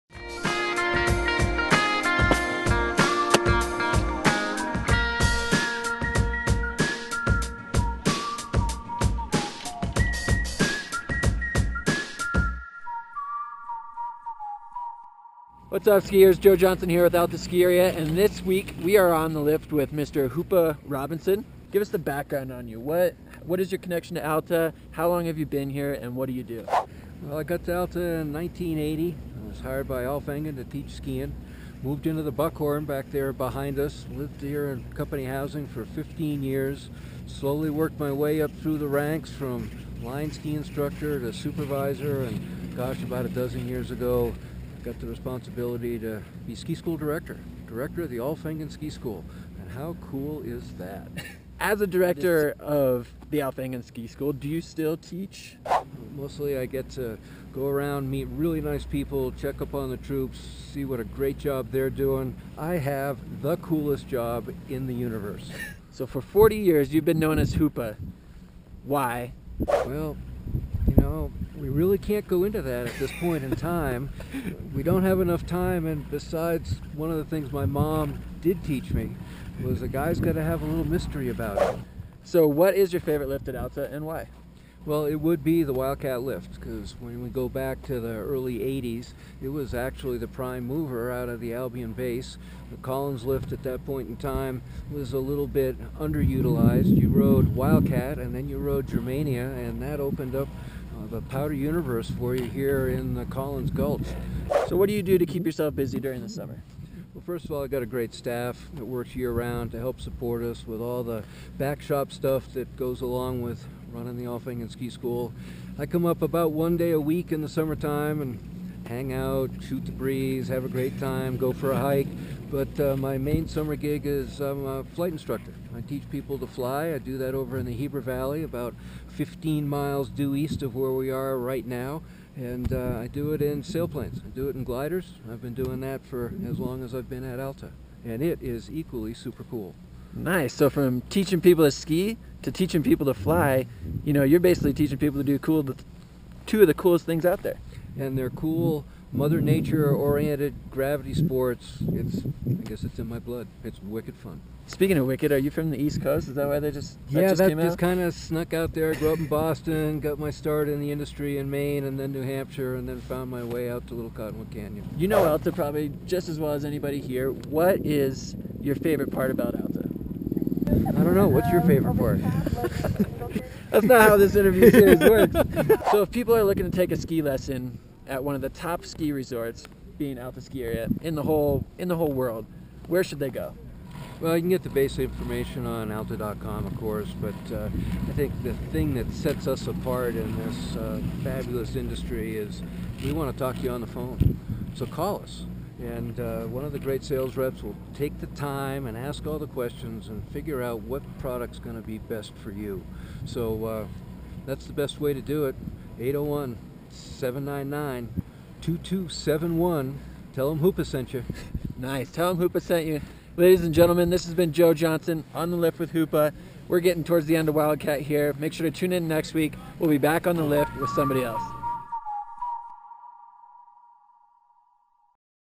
On The Lift With Hoopa Robinson
While the snow and terrain steal the show, the people and personalities that call Alta home are just as memorable. On The Lift With aims to introduce you to those people and what makes them tick.

This week, we took a seat with the Director of the Alf Engen Ski School, Hoopa Robinson who claims to have, "The coolest job in the universe."

A big thank you to Timothy Jones, TJ Videography, for his masterful work on the "On The Lift With" introduction.